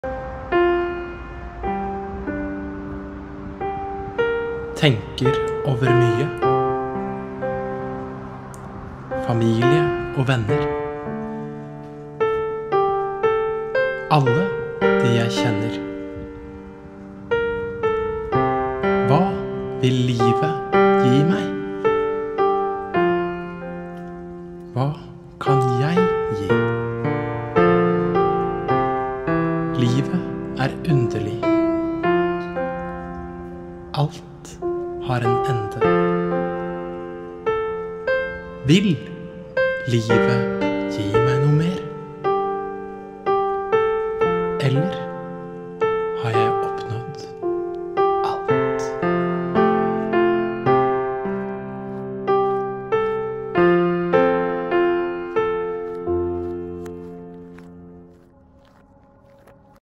Dikt: Tenker over mye
Leser opp et gammelt dikt jeg skrev for over 15 år siden. Mye har skjedd siden den gang.